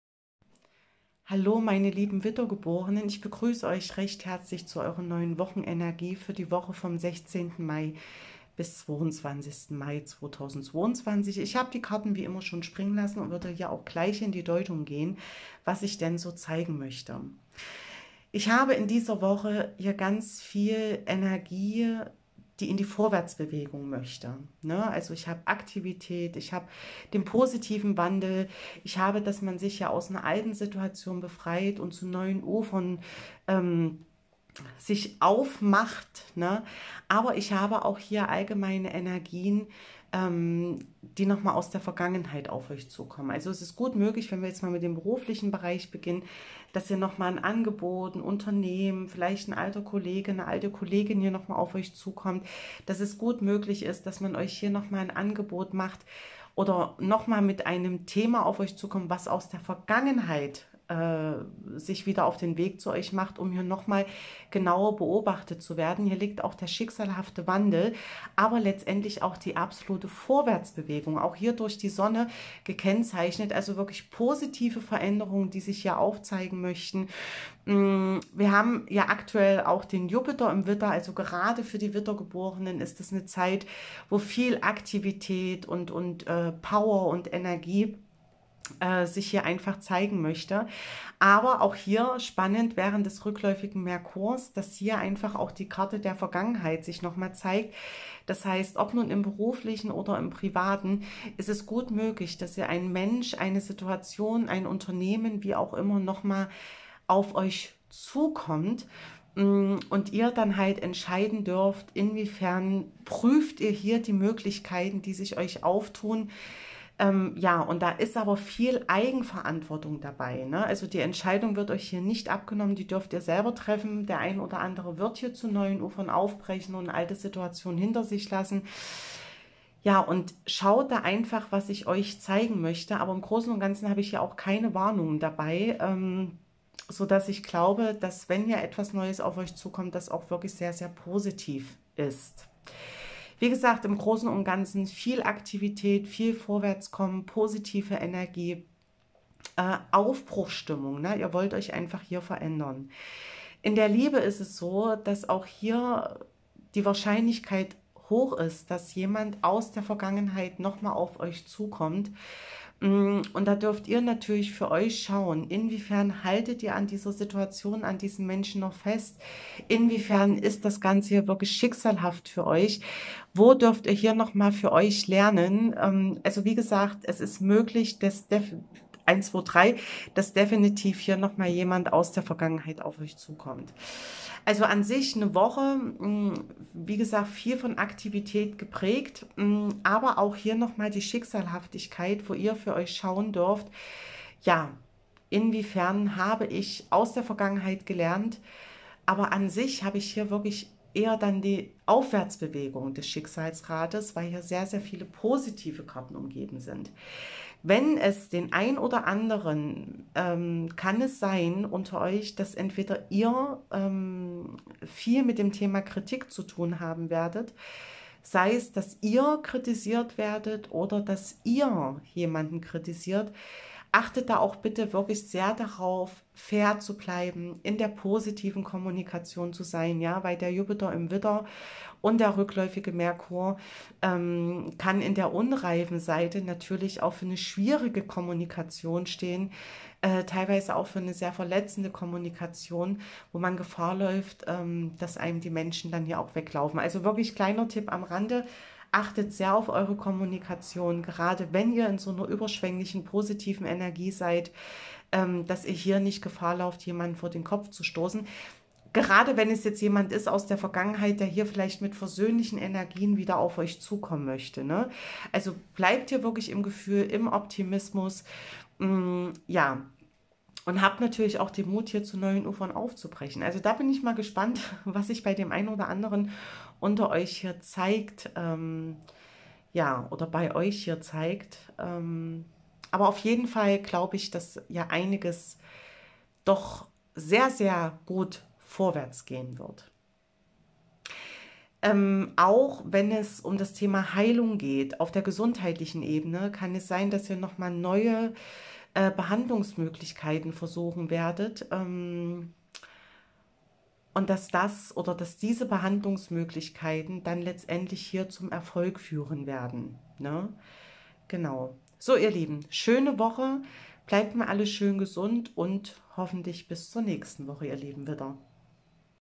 WIDDER ❤️WOCHENENERGIE VOM 16.5.BIS 22.5.2022
Verwendete Karten:

Gilded Reverie Lenormand Ciro Marchetti Königsfurt Urania Verlag

After Tarot Lo Scarabeo

Universal Waite Tarot Deck U. S Games Systems

Lenormand Weiße Eule ISBN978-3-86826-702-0 Königsfurt Urania

Bitte bedenkt, dass es allgemeine Energien sind. Sie können, müssen aber nicht auf Euch zutreffen.